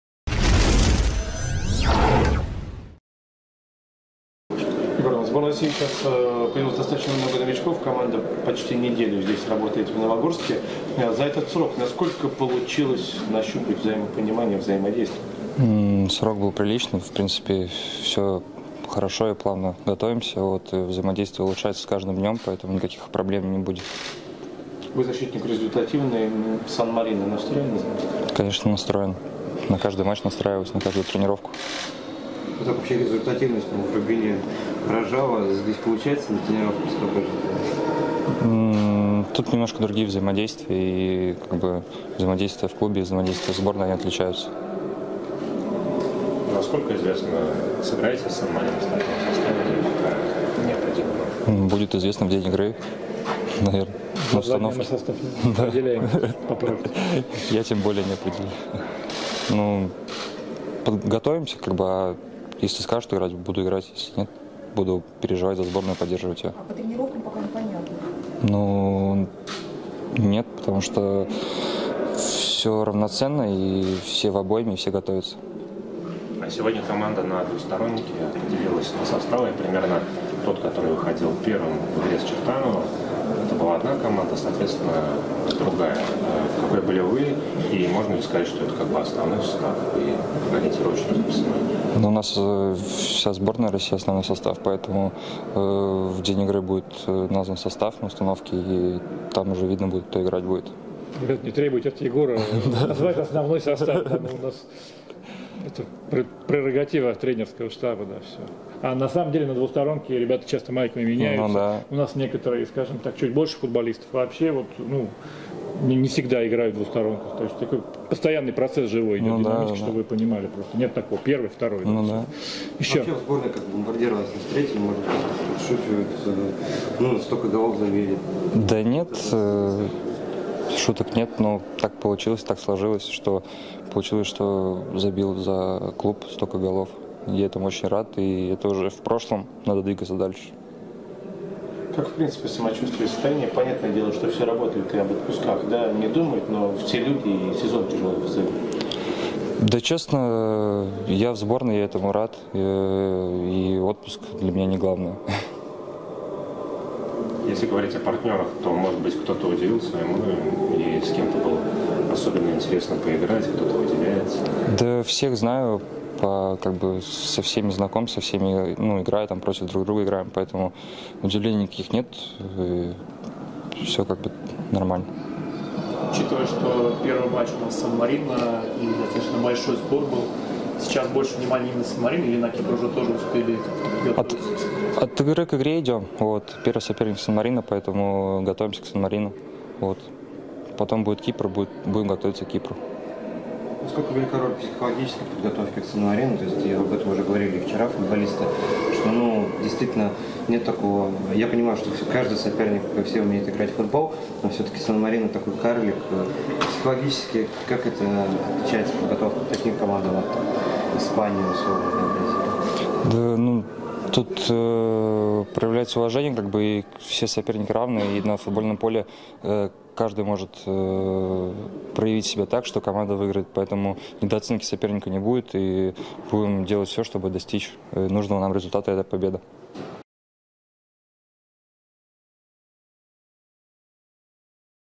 Егор Сорокин: «Для меня отпуск – не главное» l РФС ТВ
Защитник сборной России Егор Сорокин о командных взаимодействиях, своей результативности и ближайших матчах.